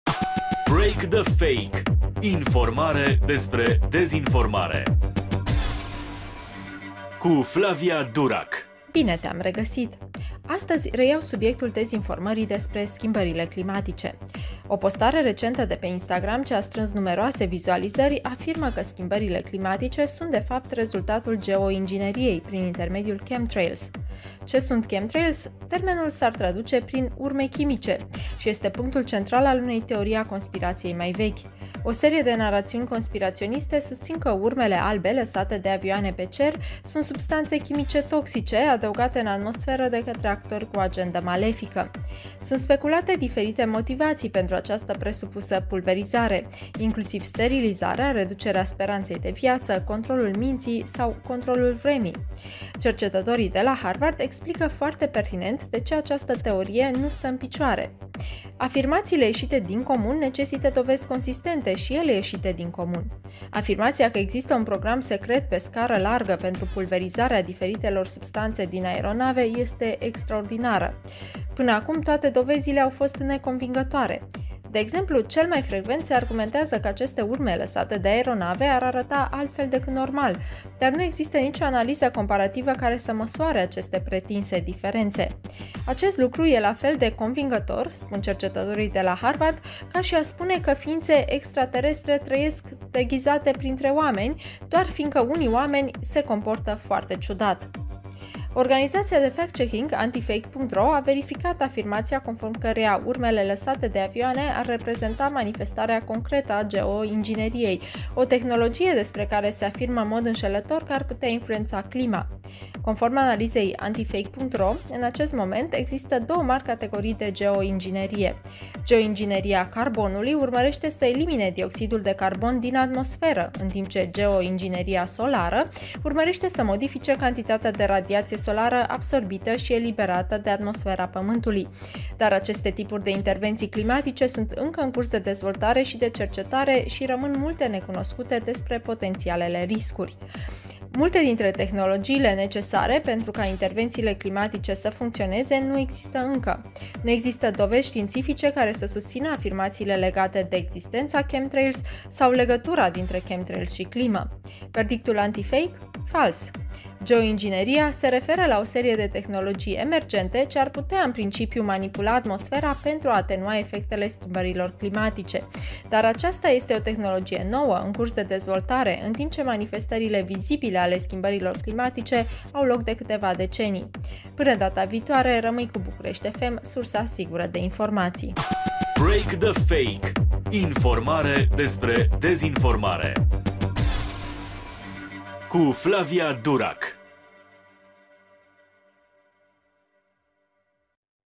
Fake: geoingineria, cauza schimbărilor climatice
O postare recentă de pe Instagram ce a strâns numeroase vizualizări afirmă că schimbările climatice sunt de fapt rezultatul geoingineriei, prin intermediul chemtrails.